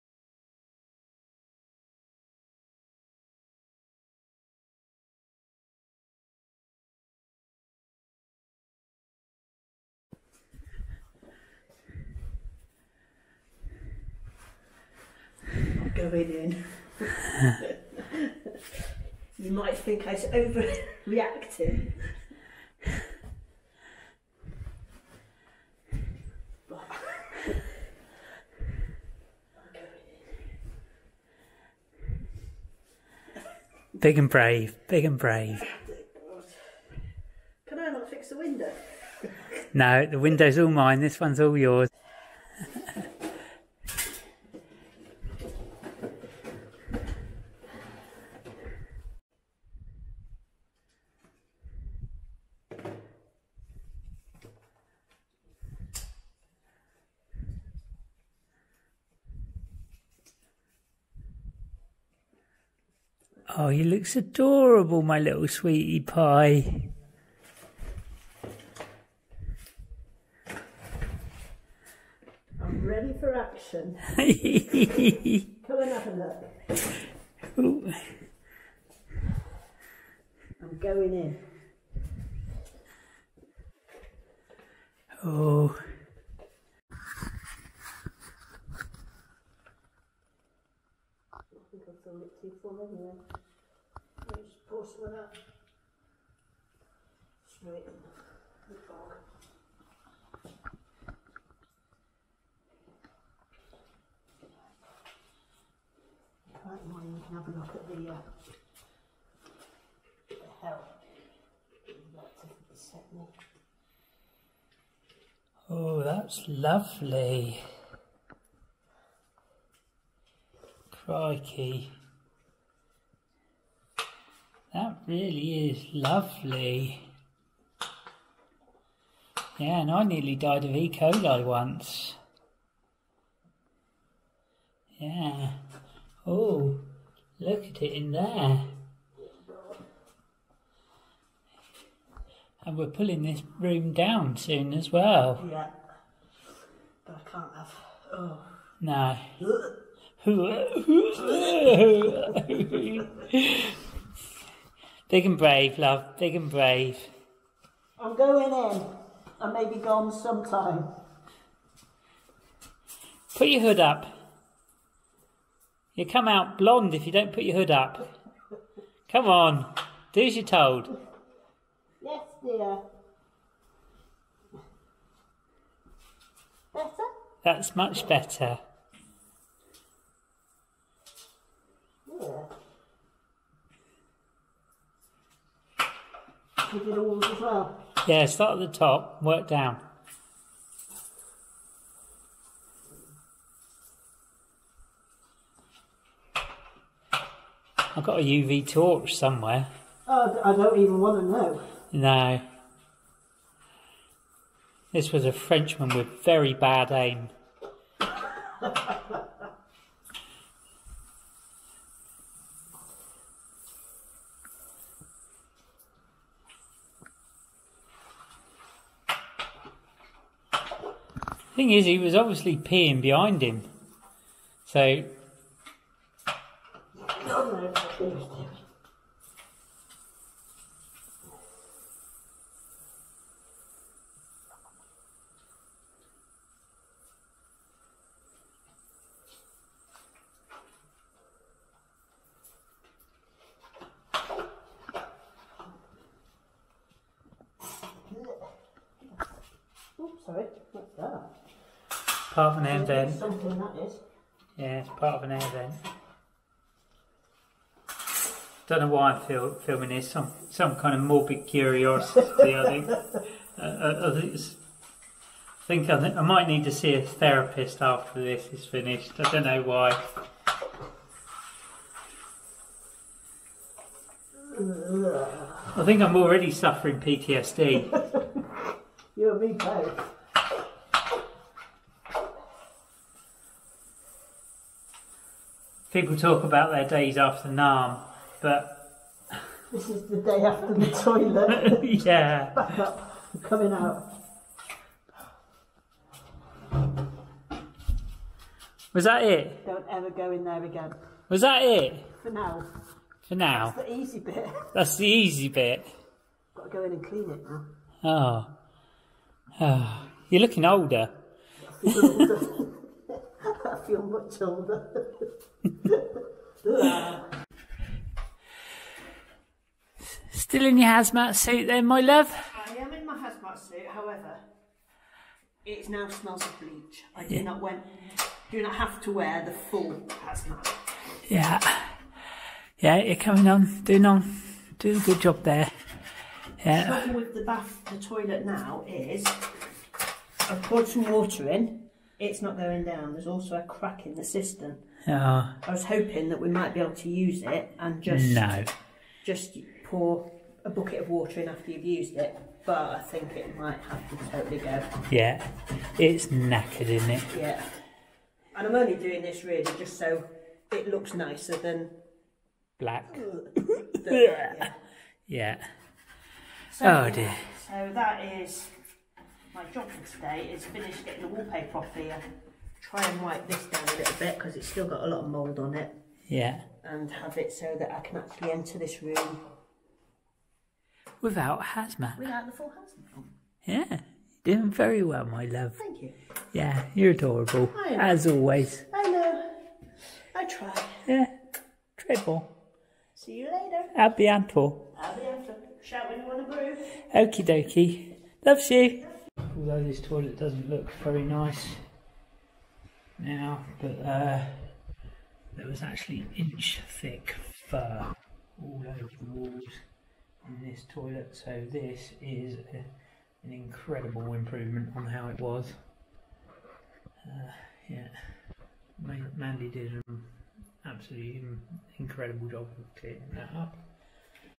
Hostile Environments and Hazmat Suits
Our French Renovation
My wife is a brave lass. She has served her time in the army and has visited a few war zones. The downstairs Toilet was to be her biggest challenge yet. Left abandoned for decades the mold was inches thick and the ammonia smell was nauseating.

After buying our house we subsequently learnt it had been on the market for at least a couple of years. Inside was piled deep in discharged clothes, toys, cooking utensils, boxes from decades past. Mold, dust and cobwebs were thick on many walls. The internal pictures alone would have put 95% of most potential buyers off from visiting this house. Luckily, there wasn't a picture of the toilet as we may not have visited either!

The cistern was cracked, the fosse septic or septic tank location was unknown and was probably unusable. The cold water pipe to the toilet and sink was cut. There was no hot water to the sink. There was a dusty glass window that looked directly into the kitchen area.

Even though it was unused the smell was quite special. The only thing to do was to strip the walls of wallpaper and tiles. Remove the sink and replace the toilet. Every surface needed to be soaked for days in bleach and sanded with a wall sander.

This room will be coming down. It is only temporally being used until we can get either bathroom running. The sewage outlet will however be where the washing machine will sit so the pipework is in a convenient position.